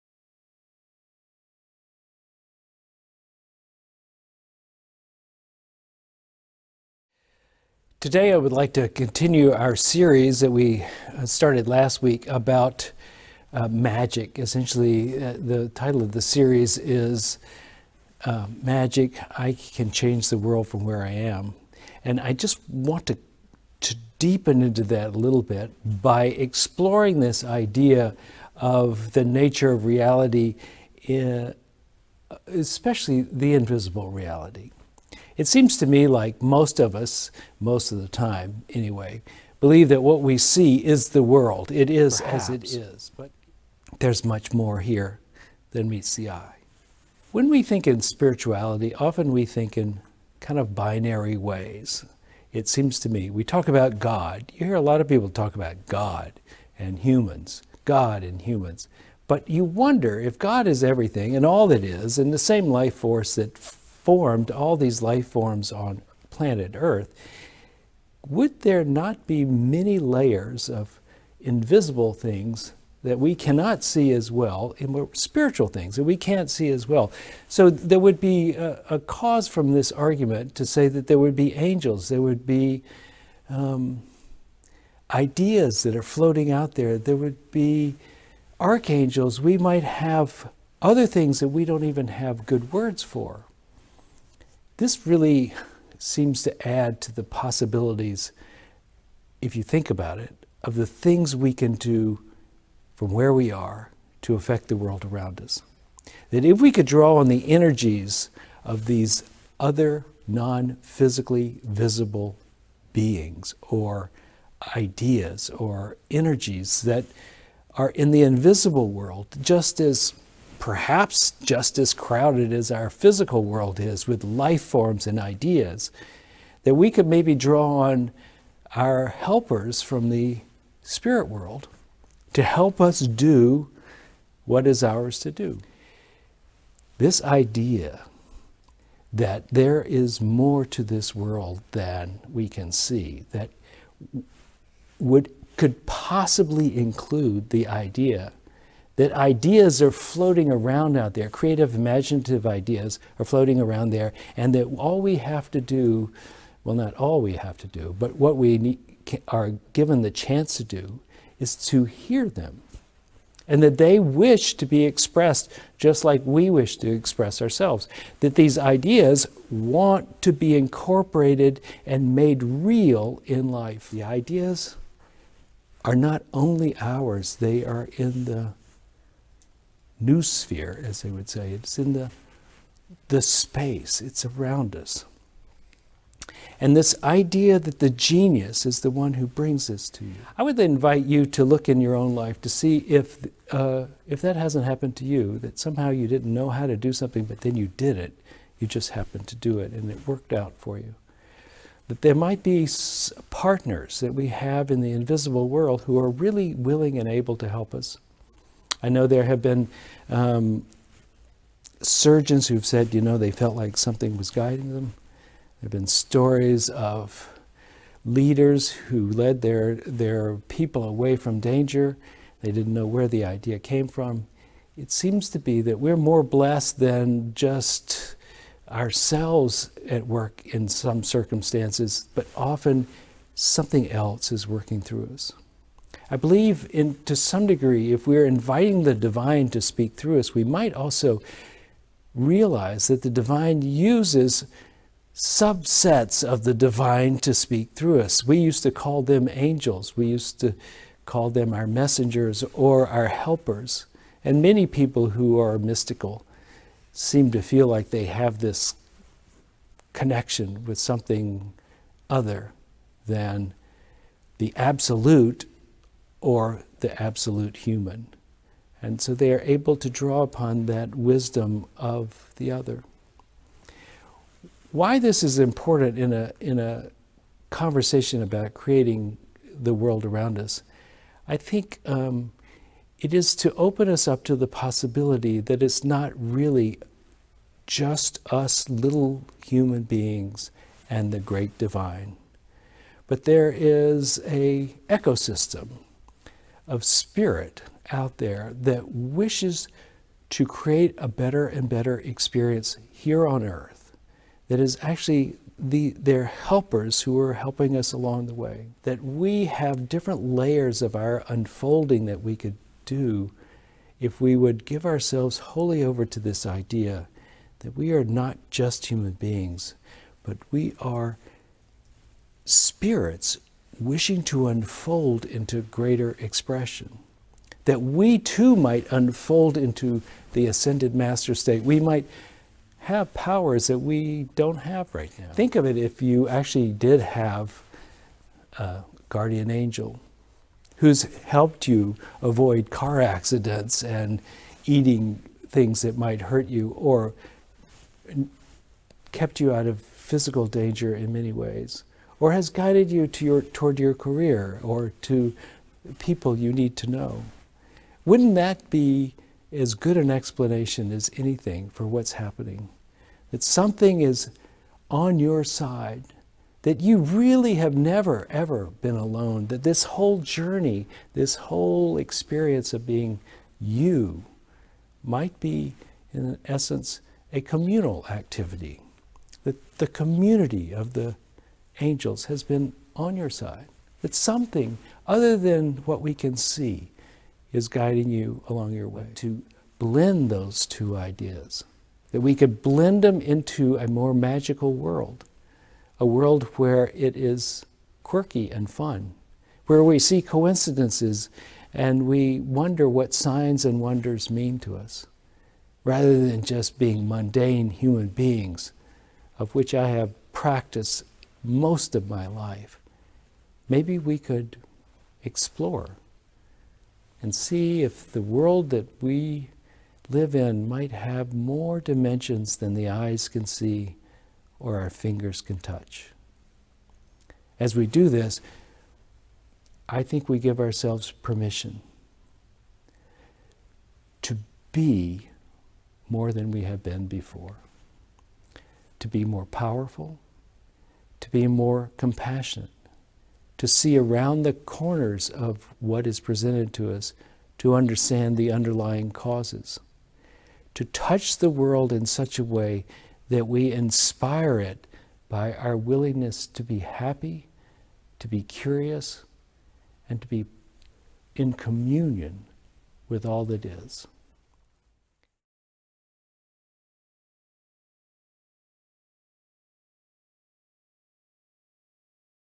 Sunday in 7 - Explore the Mystery | Rev Neusom | Unity of the Triangle | July 22 2020
The possibilities of what lies beyond our vision and understanding are endless.  Guardian angels, archangels, acsended masters, messengers...whatever form they take, there is surely some benevolent celestial intermediaries between us and God.